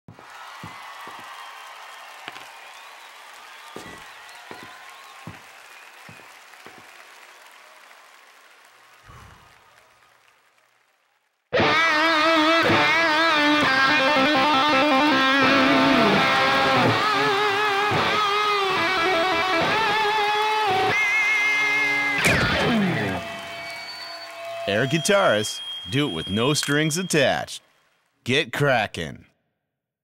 Air Guitarist Gets Crackin'
2009 wonderful pistachios national tv commercial contest winner